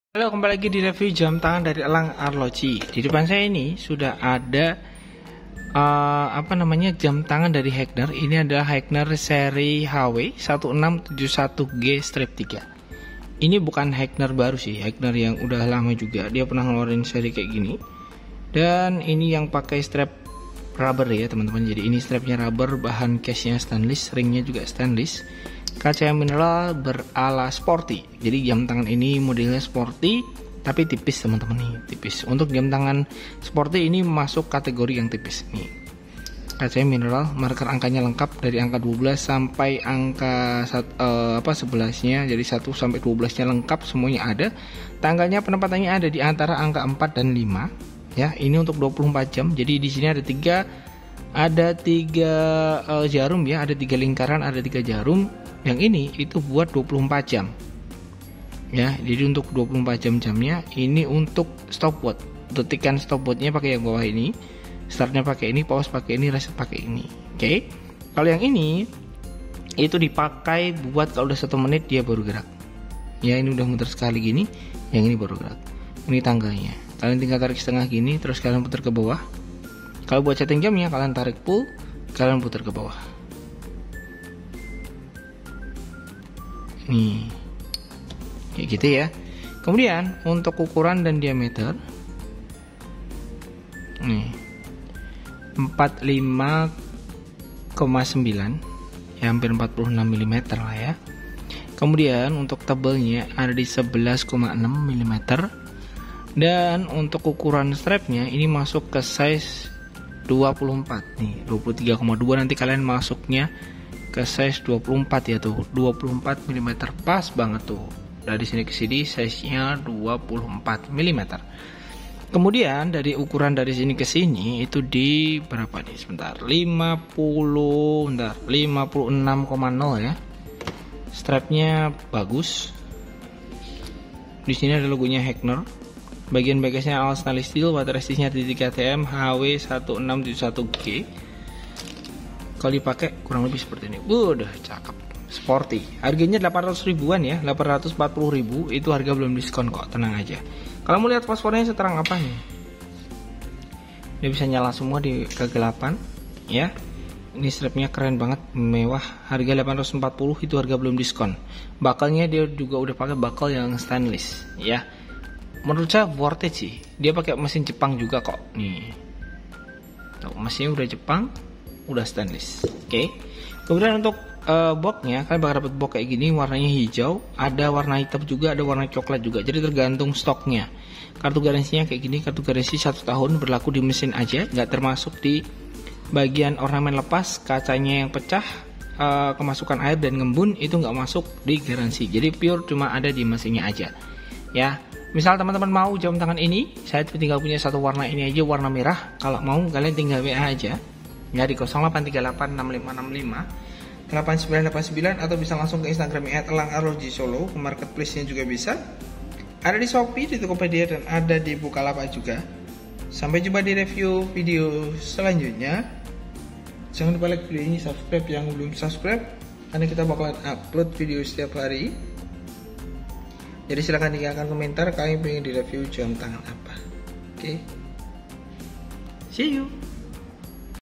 HEGNER HW 1617G-3 : JAM TANGAN HEGNER MODEL SPORTY HARGA MURAH TERBARU (2024)
REVIEW DAN DETAIL JAM TANGAN HEGNER HW 1617G-3 TERBARU.

Belanja Online?

Just contact =
ELANG ARLOJI
Jl. Kebangkitan Nasional No 34
Solo 57141